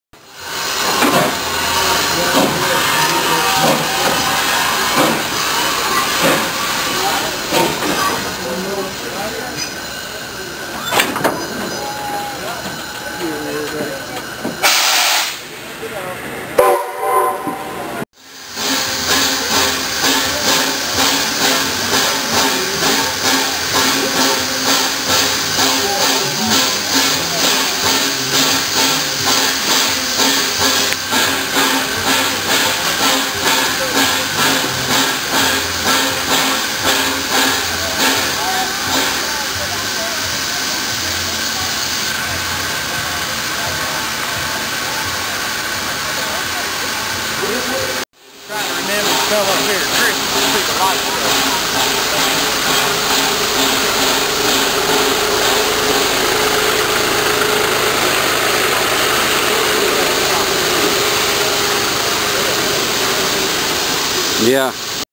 Steam Engine Train Video
A video of a 1942 50 ton class BSI steam locomotive pulling into its station in North Carolina. This engine was used in both the shipyards in Maryland and the US Naval Shipyard in Norfolk, Va. The video was filmed at the thrasher reunion in Denton, NC where the engine spends its remaining days after being purchased from the US government and restored. You can view the still images of this and other trains in black and white new images are added regularly enjoy the video.